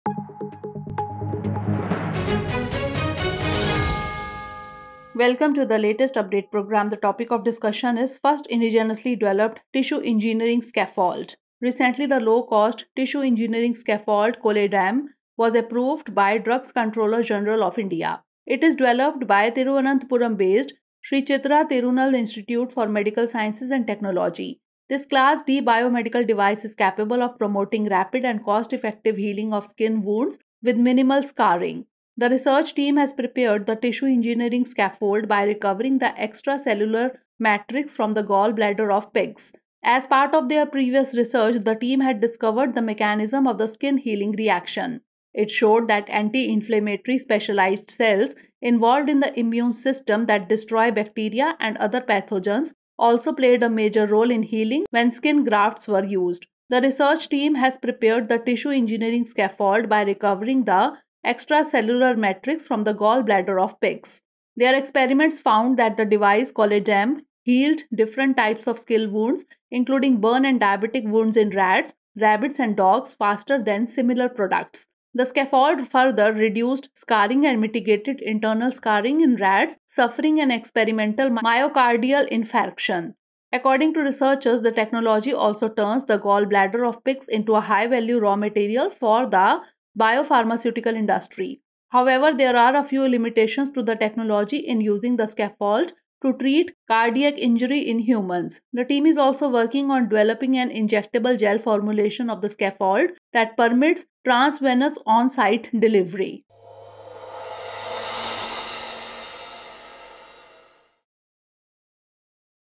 Tissue engineering scaffold : Latest update | Drishti IAS English
Recently the Low-cost tissue-engineering scaffold Cholederm, developed by Thiruvananthapuram-based Sree Chitra Tirunal Institute for Medical Sciences & Technology was approved by Drugs Controller General of India (DCGI).